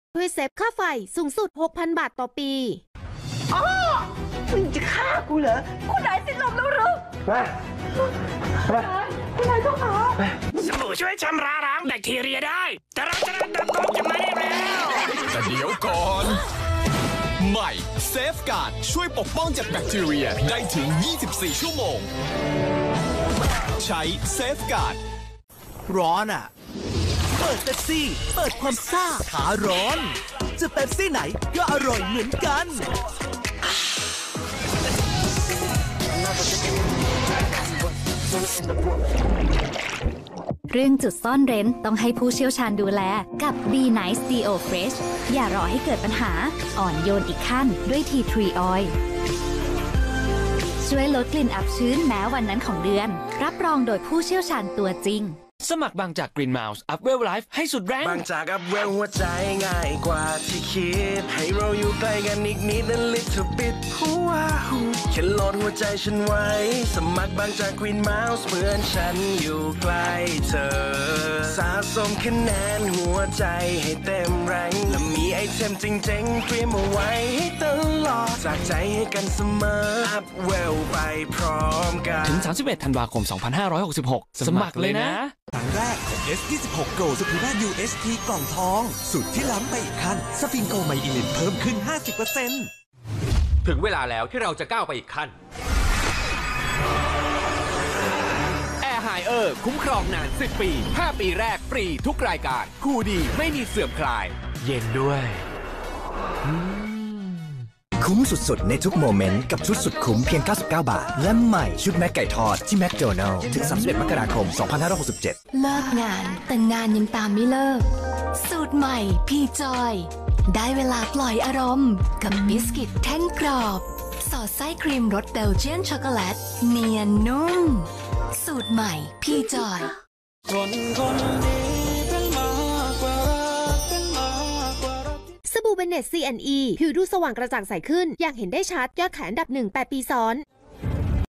HD โฆษณา 2566 - 193
โฆษณาคั่นละคร หมอหลวง ในวันจันทร์ที่ 1 พฤษภาคม 2566 ทางช่อง 3HD